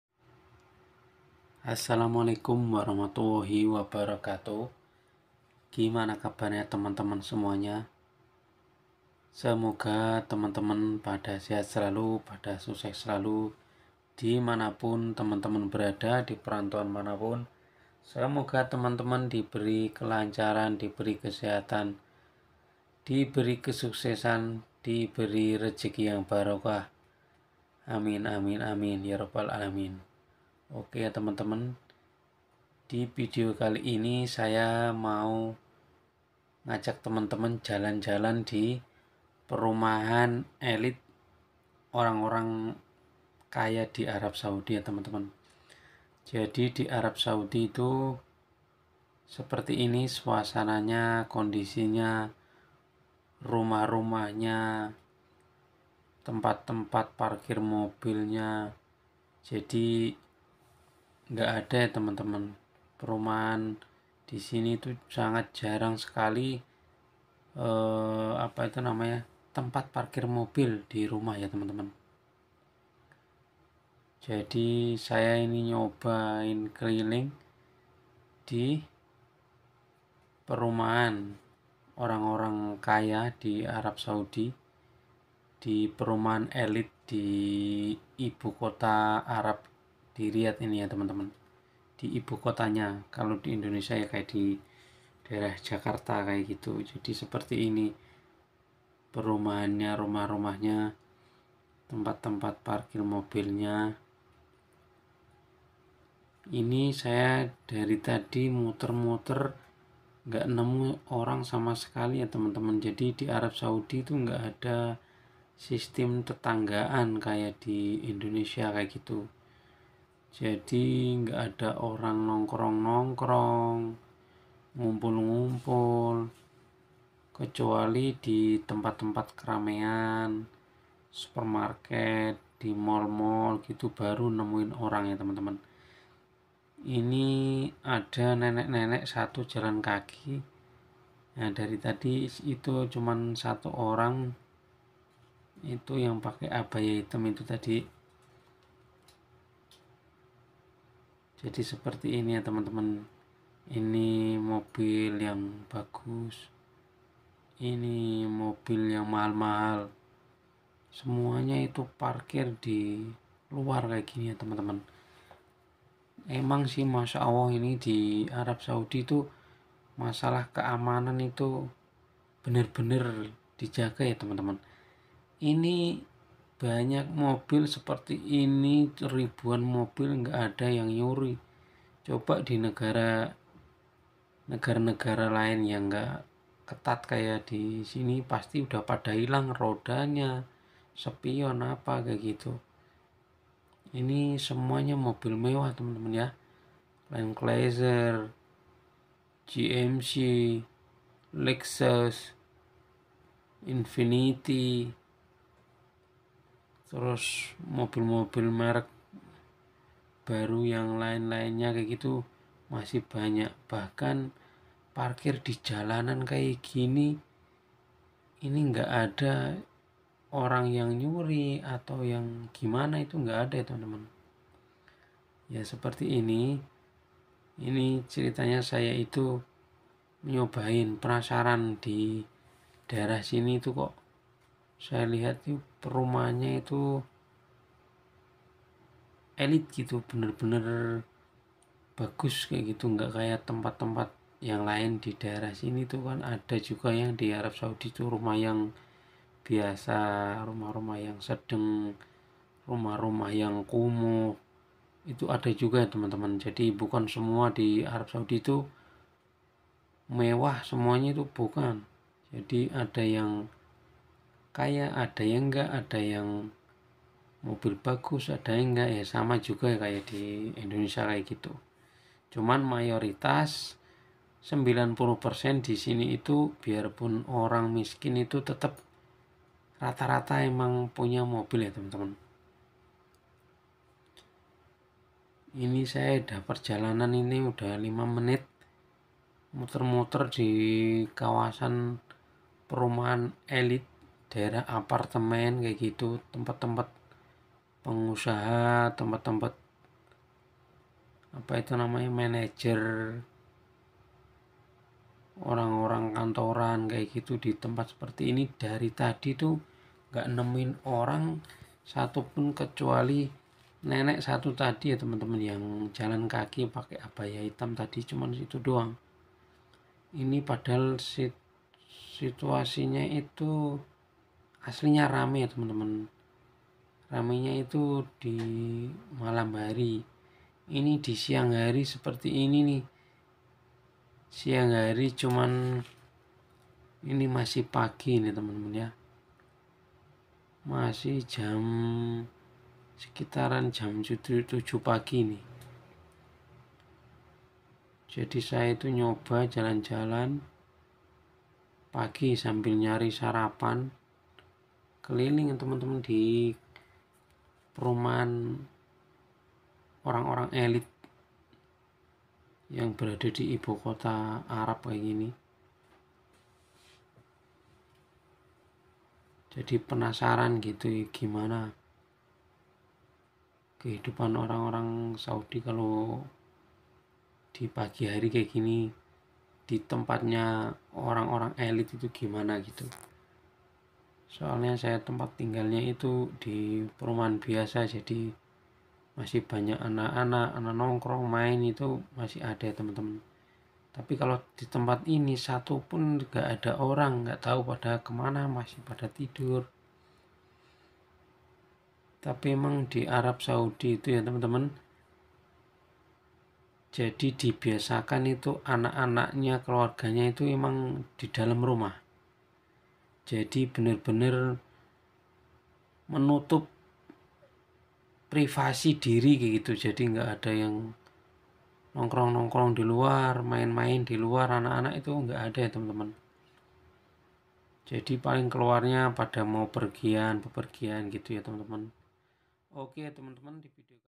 VLOG KELILING DI PERUMAHAN ELIT YANG SANGAT MEGAH DAN MEWAH TAPI SEPERTI KOTA MATI !!!
Halo semua  di Vidio ini saya lagi nyobain keliling di perumahan elit yang sangat megah dan mewah yang berada di ibukota Arab SAUDI yang seperti kaya kota mati karna GK nemuin orang satupun di luar rumah @KangBudichannel